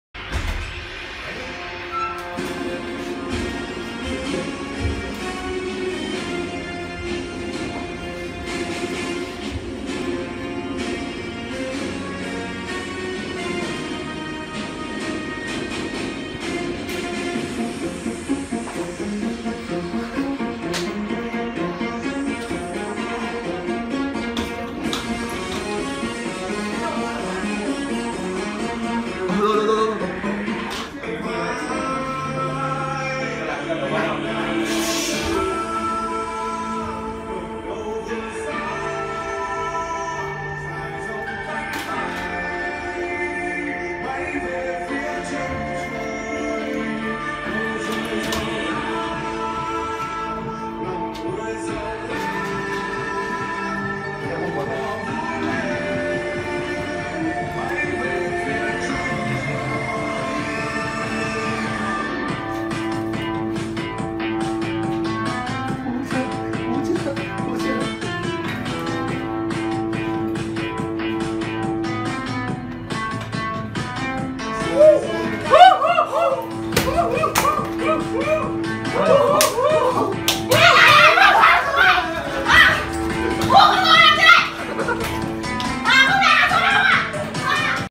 Tây Du Ký phiên bản của các bạn học viên HICADO
Thầy trò Đường Tăng đi thỉnh kinh phiên bản của học viên HICADO đã đủ làm các bạn thích thú hay chưa? Còn rất nhiều màn trình diễn những phần thi sáng tạo và đầy hài hước khác đang chờ các bạn khám phá đấy.

Địa Chỉ : Số 47, đường Trung Yên 14, Trung Hòa, Cầu Giấy, Hà Nội.